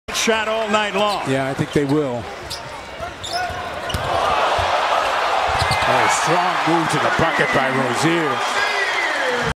NBA, playoff 2018, Celtics vs. Bucks, Round 1, Game 1, Move 5, Terry Rozier, layup
Boston Celtics - Milwaukee Bucks, Game 1, 15/04/2018, quarter 1
Terry Rozier, layup